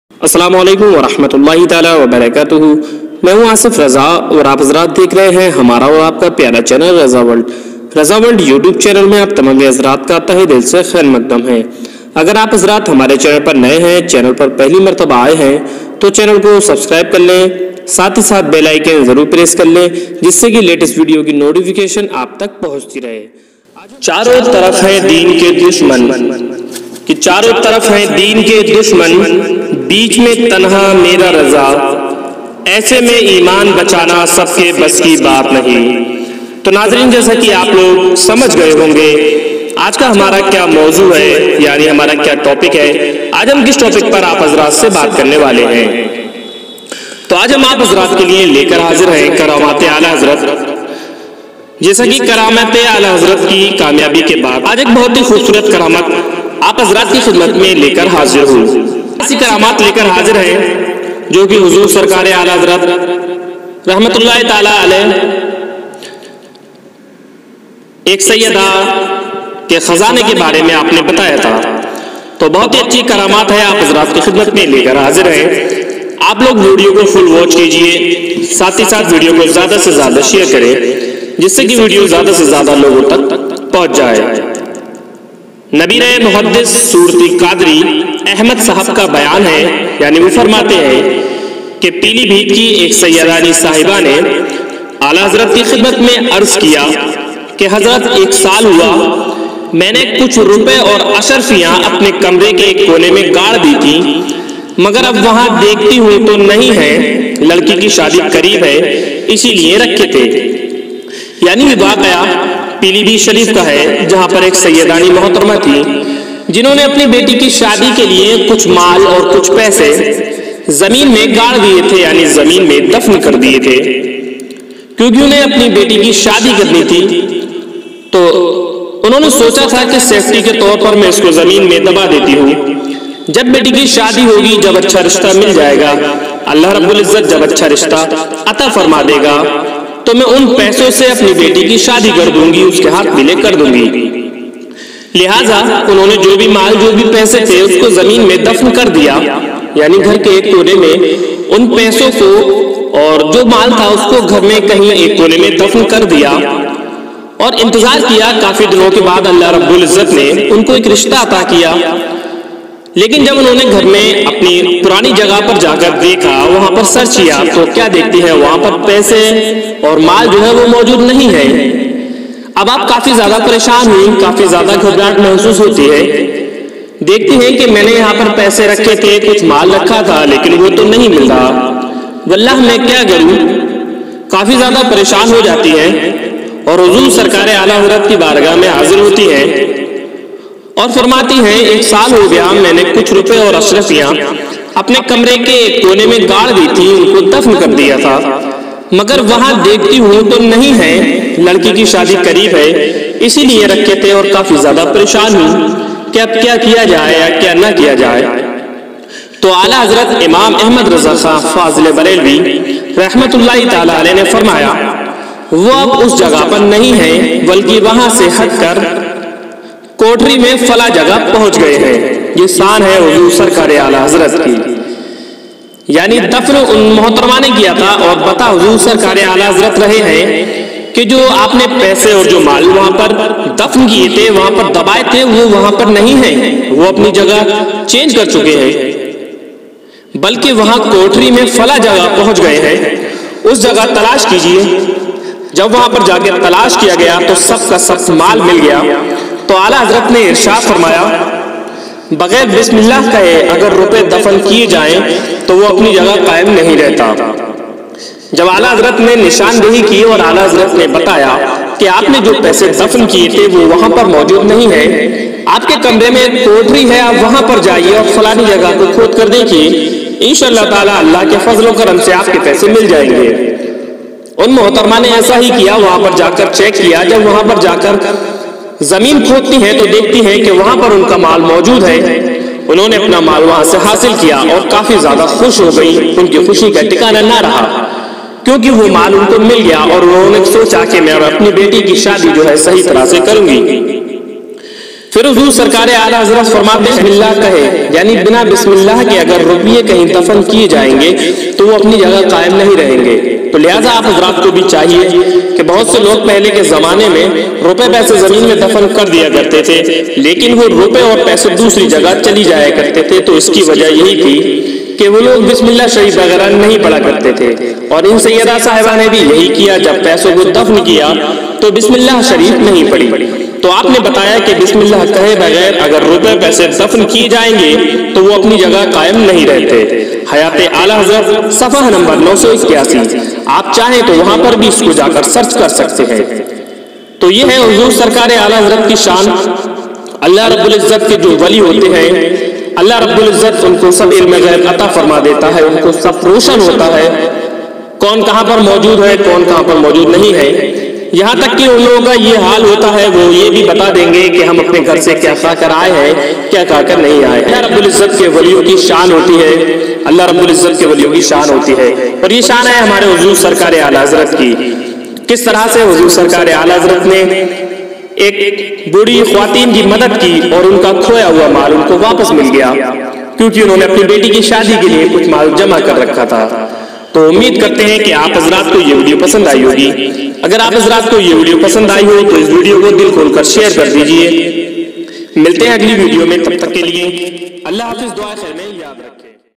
Karamat e alahazrat ऐक औरत का गुमा हुआ सोना-चांदी मिल गया
Aaj Ki video me karamat e alahazrat aap logo KO batayenge

The official YouTube channel Raza world

          (AASIF RAZA)

Raza world
Razaworld